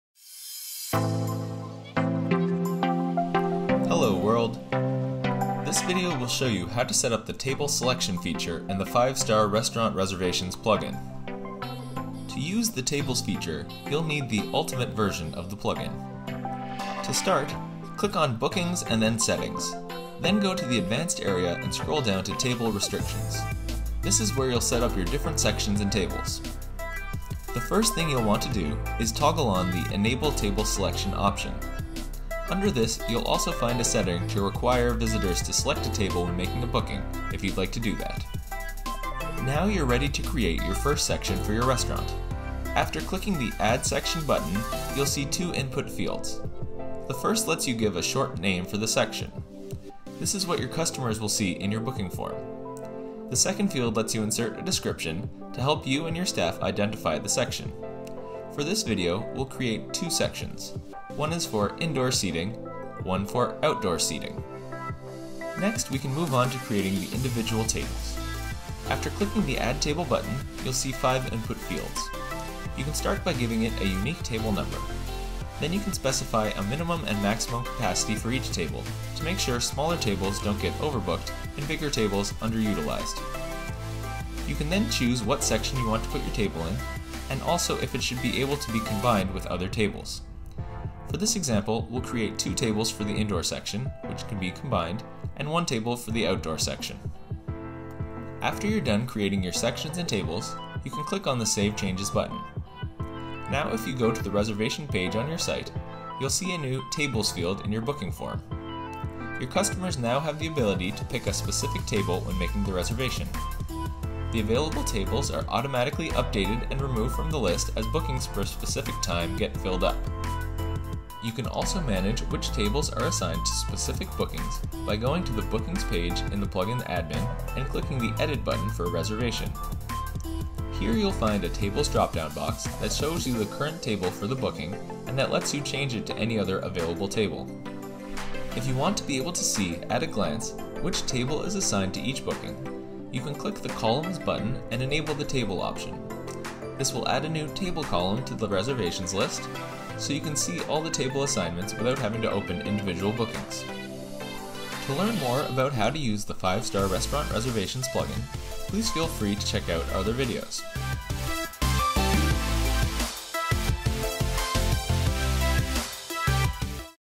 Five Star Restaurant Reservations - Tables and Sections
This video shows you how to set up and use the tables and sections features in the Five Star Restaurant Reservations plugin Ultimate version for WordPress.